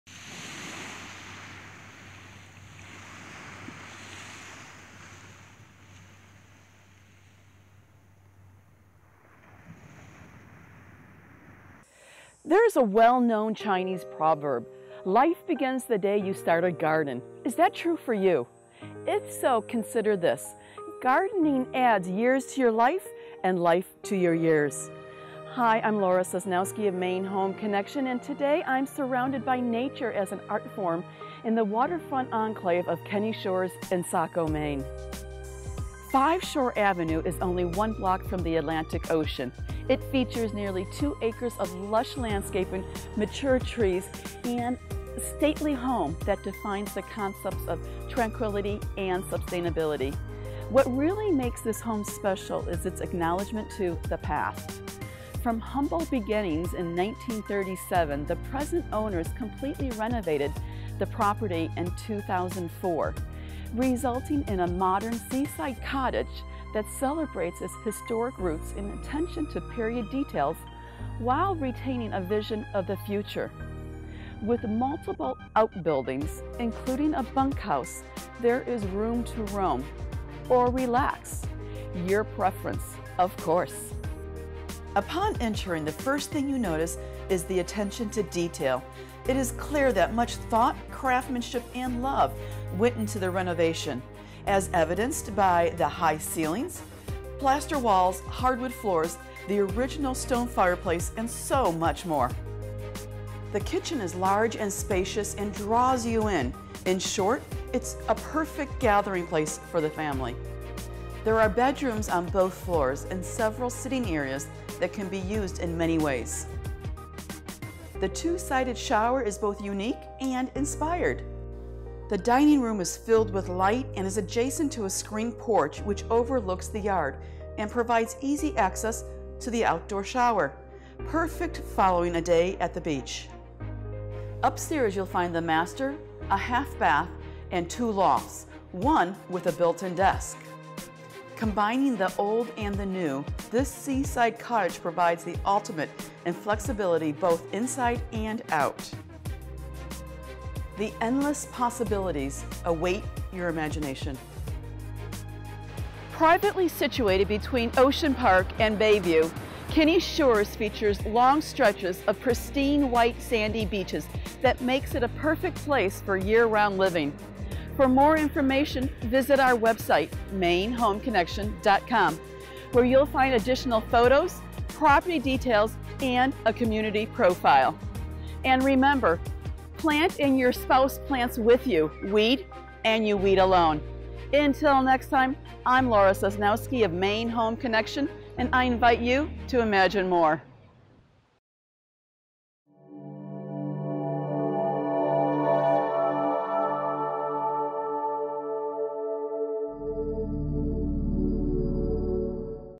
5 Shore Avenue | Kinney Shores
Gardening adds years to your life and life to your years.  Yes, when you are surrounded by nature in the secluded waterfront enclave of Kinney Shores in Saco Maine.

5 Shore Rd, only one block from the Atlantic Ocean, features nearly two acres of lush landscaping, mature trees and a stately home that defines the concepts of tranquility and sustainability.

Laura Sosnowski
Maine Home Connection